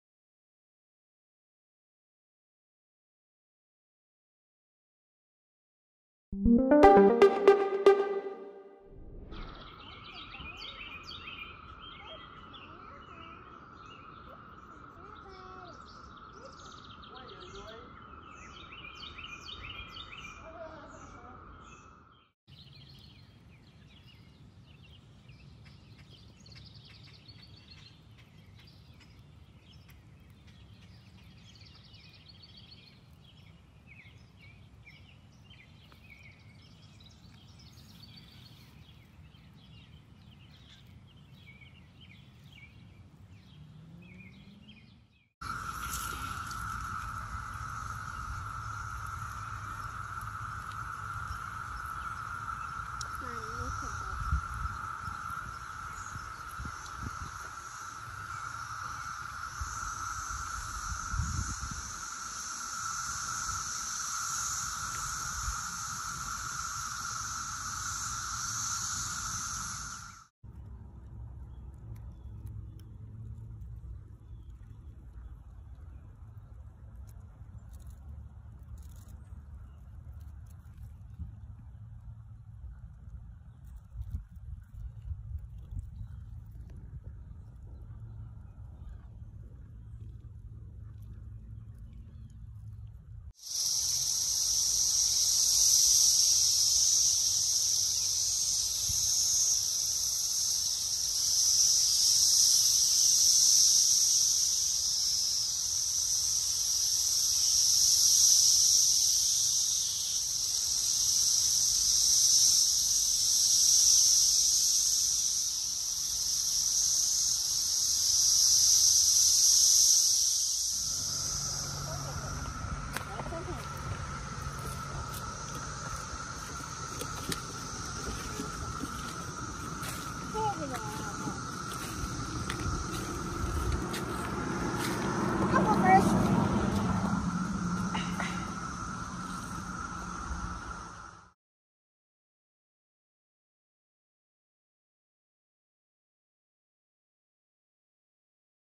Miljoenen insecten in VS kruipen na 17 jaar uit de grond om te paren | NU.nl
Eens in de zeventien jaar kruipen miljoenen cicaden uit de grond in de Verenigde Staten. De insecten hebben als larve zeventien jaar onder de grond geleefd en komen nu naar boven om te paren.